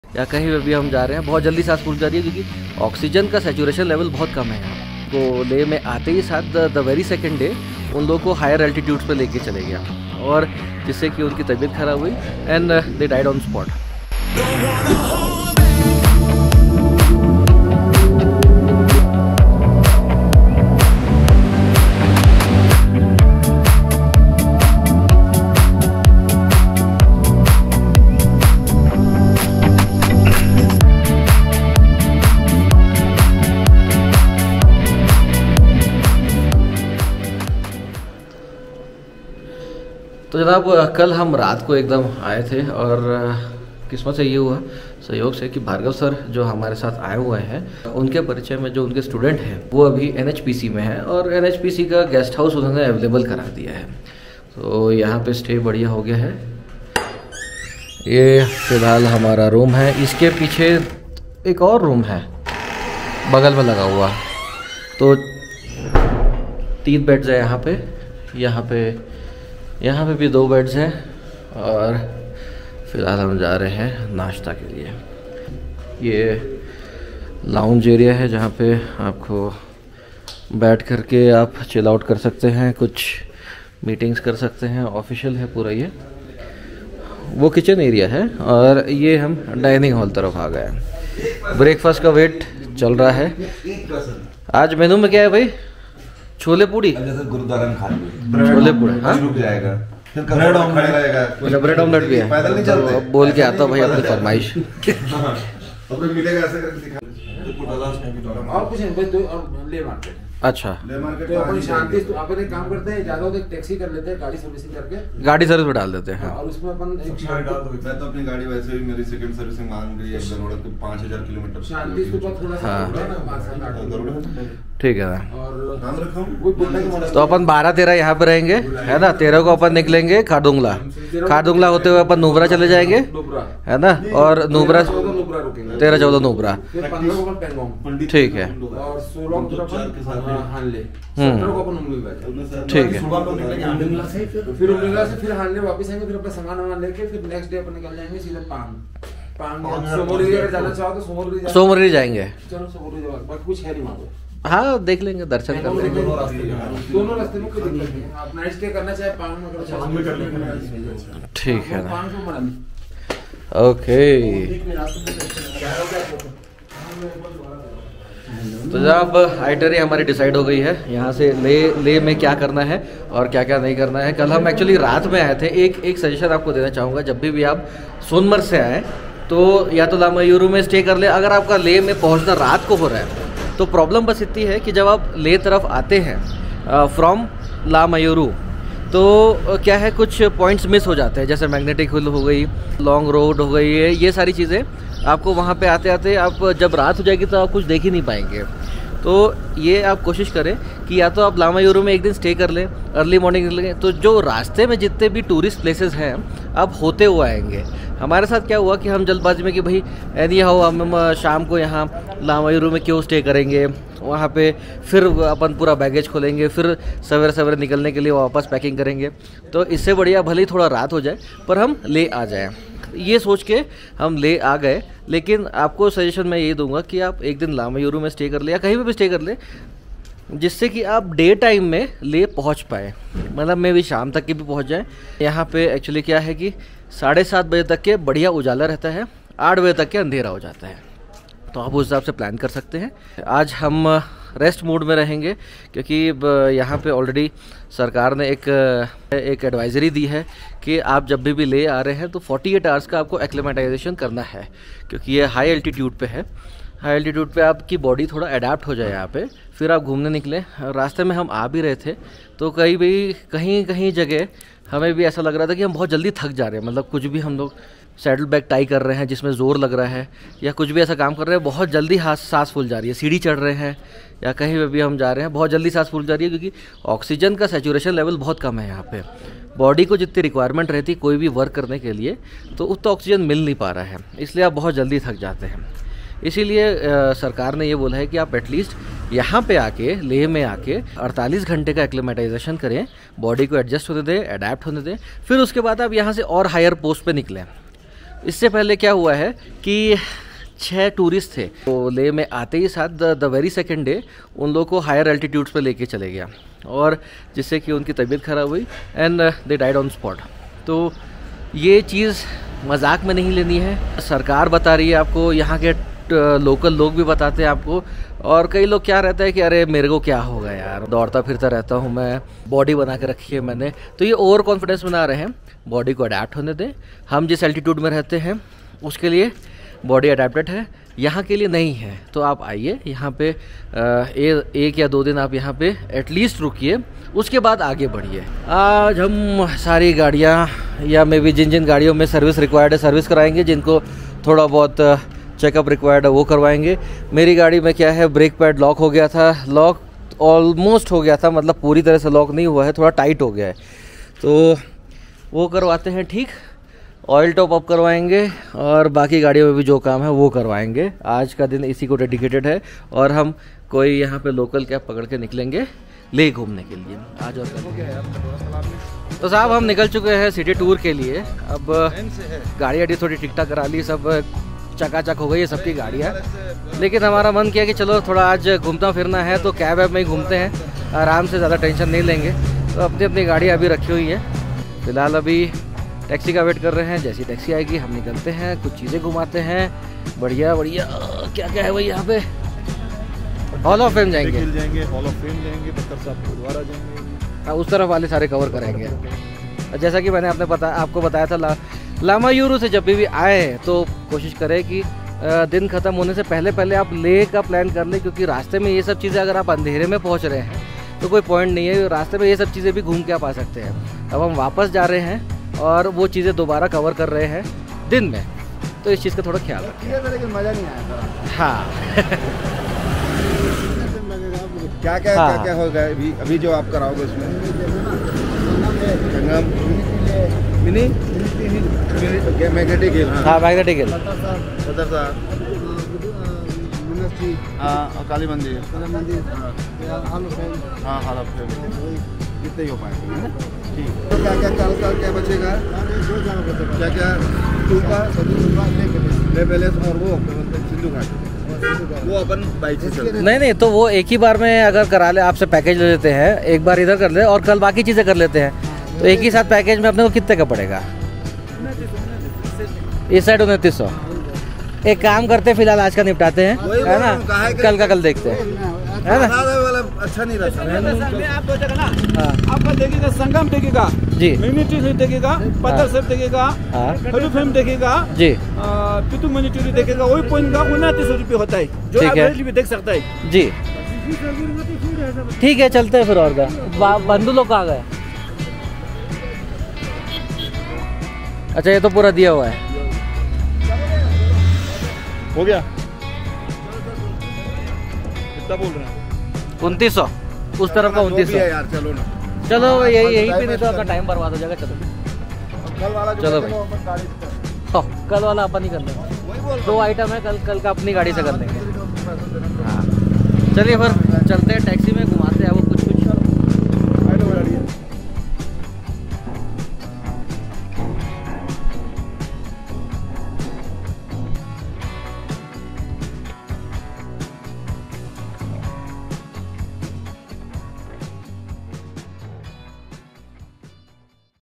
[4K] Leh: Important Points | 48 hours Acclimatization | When to reach Leh | EP-9
Why you have to take 48 hour Mandatory stay at Leh seriously which is adviced by the Government?

Gadgets I use:

1. Samsung Galaxy S22 Ultra
2. Insta 360 One R
3. Go Pro Hero 9

00:00 - Intro
00:38 - NHPC Guest House, Leh
01:48 - Planning ahead
02:54 - When to arrive at Leh
04:03 - 48 Hours Acclimatisation
04:20 - Symptoms of Acute Mountain Sickness
04:58 - Tourist death at Leh/ Ladakh
05:30 - Today's Plan
07:03 - Taxi/ Cab Sightseeing and its Itinerary with prices
08:38 - Sneak peak at next episode